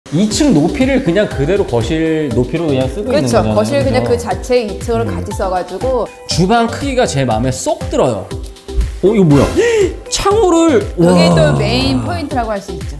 [거래종료]강원도 춘천 시내근접 자연풍경이 멋진 구봉산 인근 전원주택
본영상은 거래를 위해 제공되는 것이 아닌,
주택의 트렌드 및 동향을 쉽게 알려드리기 위한
참고 자료입니다.

영상에 출연하는 중개사는 콘텐츠의 원활한 진행을
위해 출연자로 함께 참여하고 있습니다.